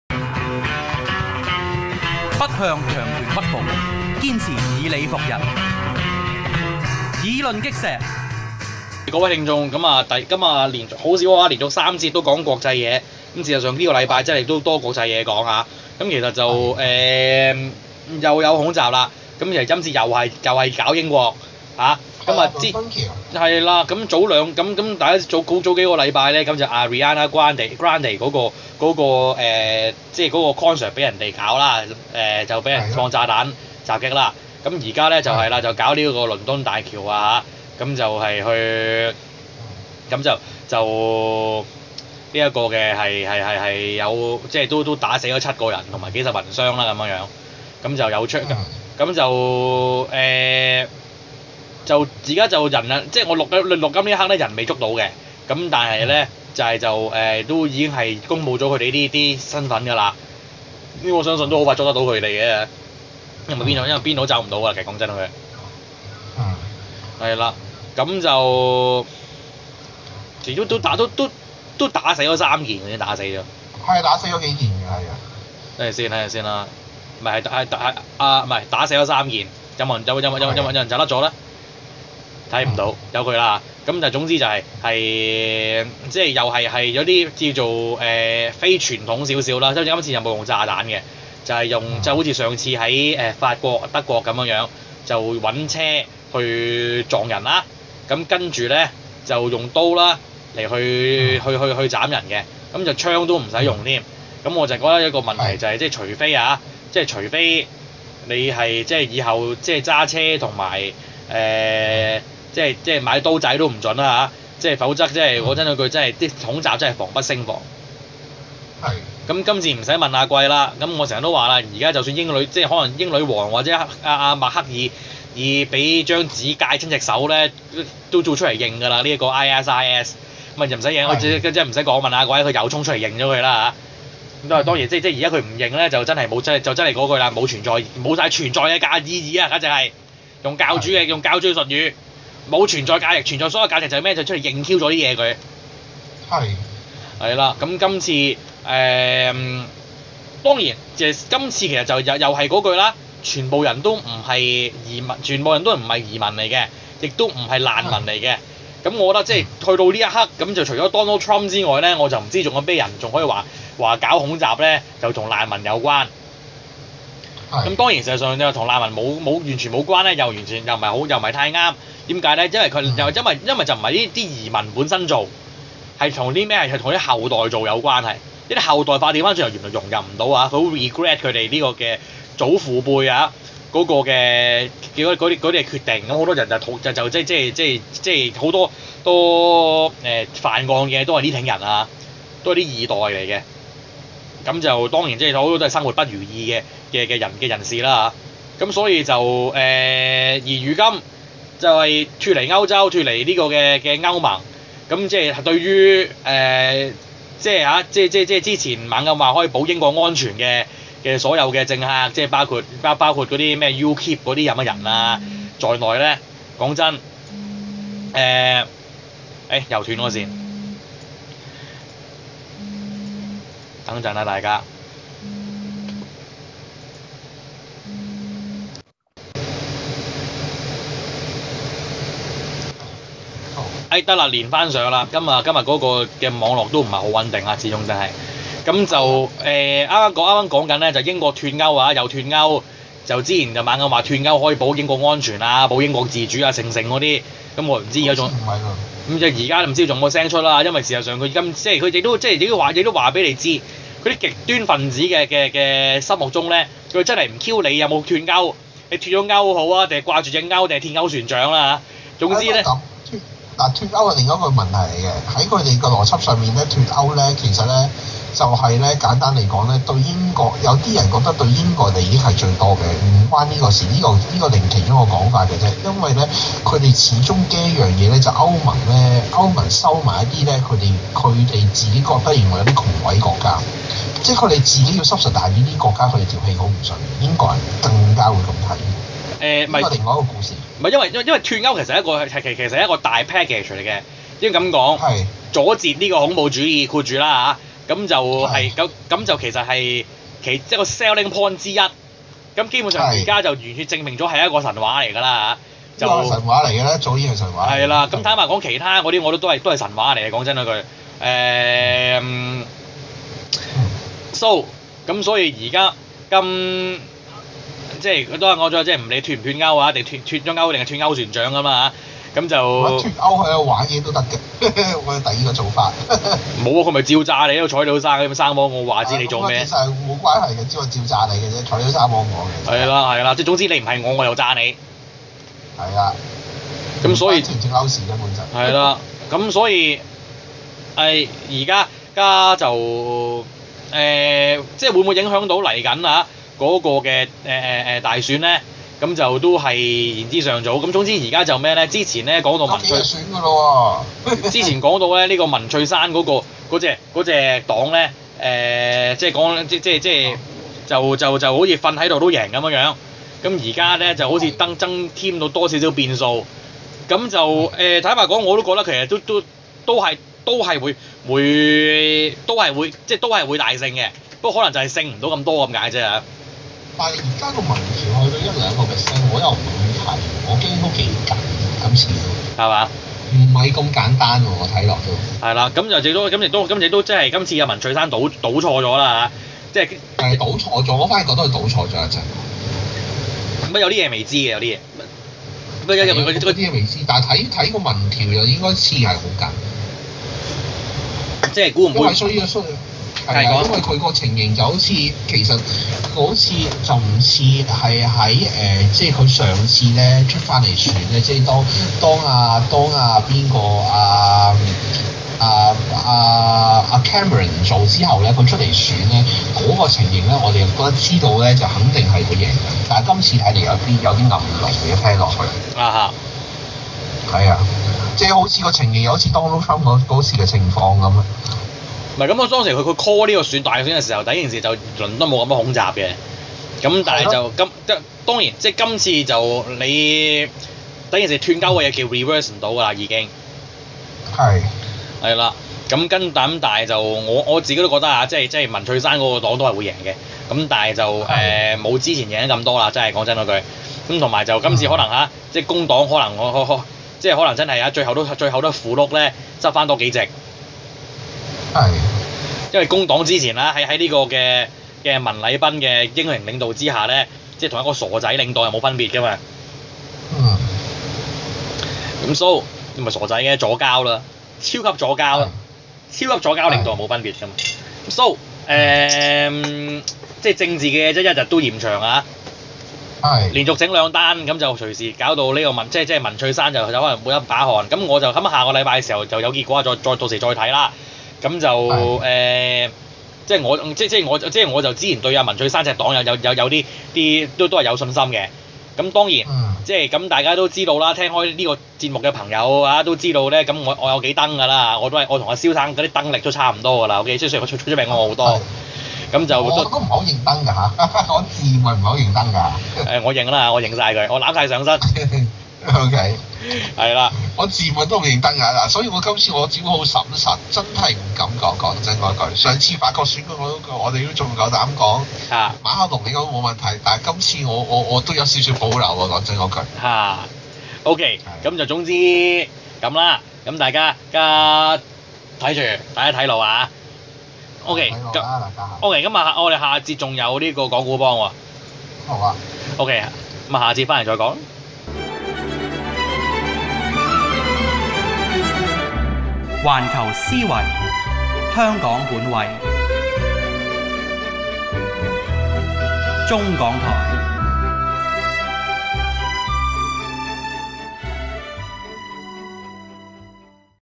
英國脫歐無法避免恐襲/ISIS的生存意義就只有承認襲擊？/預測大選保守黨、文翠珊勝出機會較大〈議論擊石〉170606 5/5
主持：Otis 阿邦

1. 英國先後發生恐怖襲擊：曼徹斯特演唱會、倫敦橋
2. ISIS的生存意義就只有承認襲擊？
3. 英國脫歐無法避免恐襲
4. 恐襲令英國國會大選添變數，預測保守黨及文翠珊勝出機會較大

‪‎忠港台‬ ChungKongChannel
環球思維 香港本位
Global vision powered by local passion